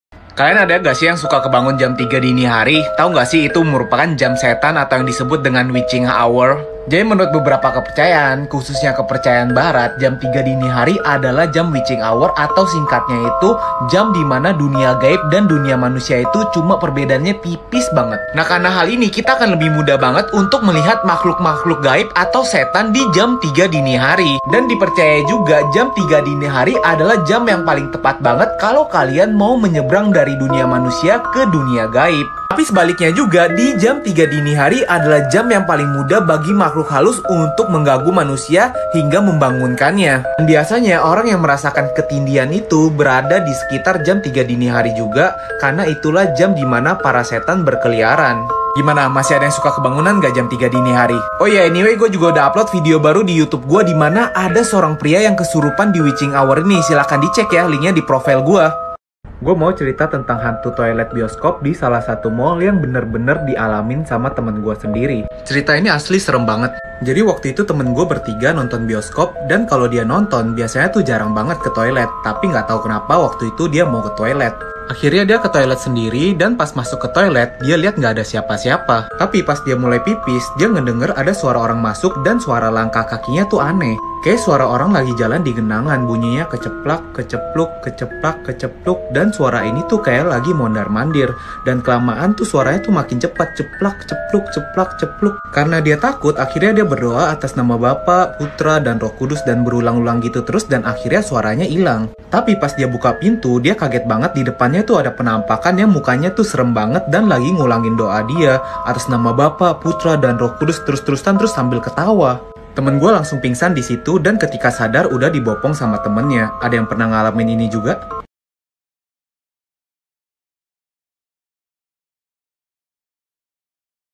LIHAT ROBLOX EDITS SAMBIL DENGERIN CERITA SERU DAN SERAM‼️😱
HALOO...guyss ketemu lagi dengan kita video ini video edits roblox dengan sensasi ngedengerin cerita seram dan seru di tonton ya guys🥰🥰😘

sound : willykun